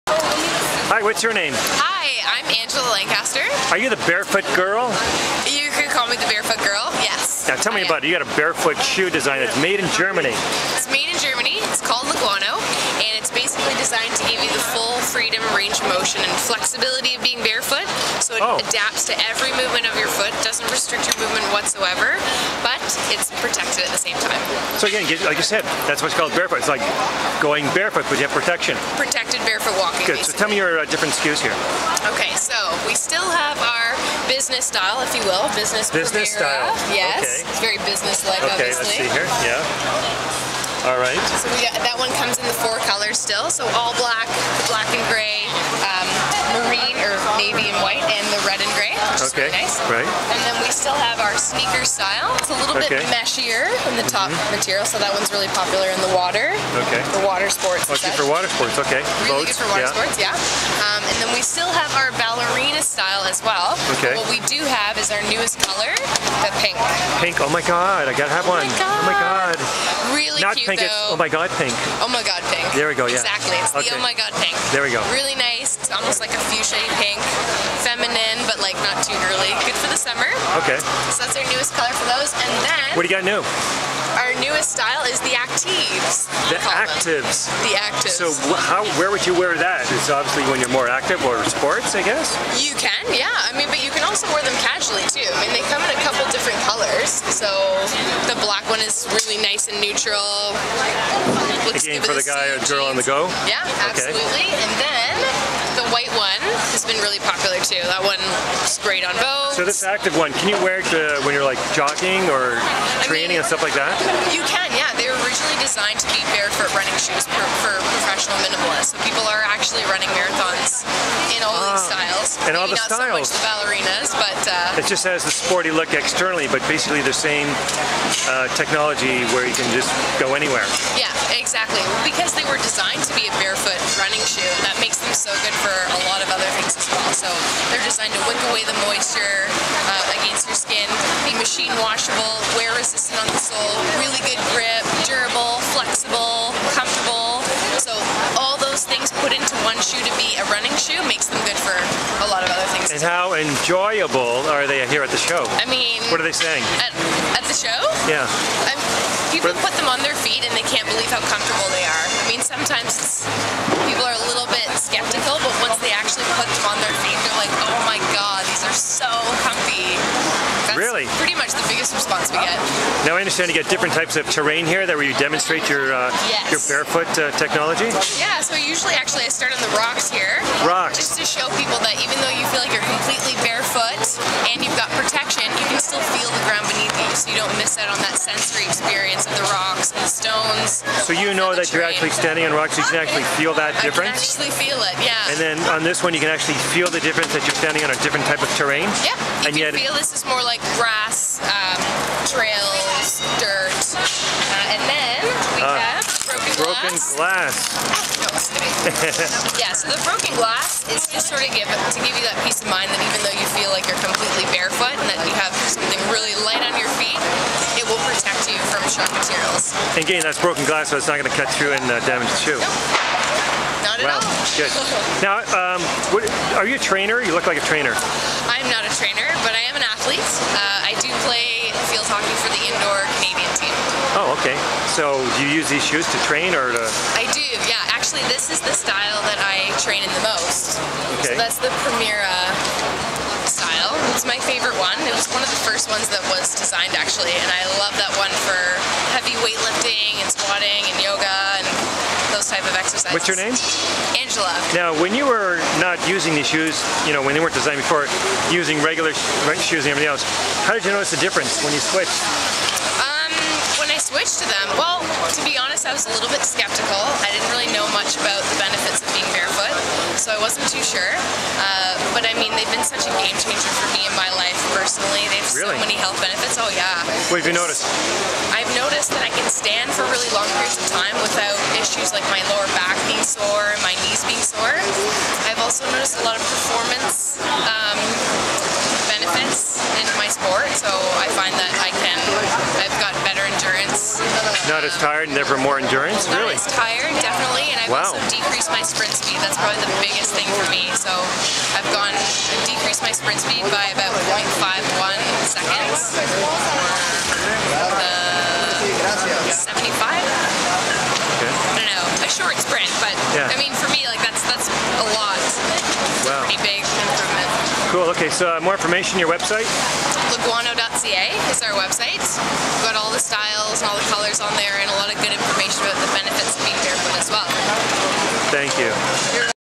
barefoot shoe design from germany @ green living show 2016
step into a radical shoe design from germany.
it bends with you to adapt to every step but protected even on glass.
there are several cool colours to choose from and designs.

@ Metro Convention Centre, Toronto.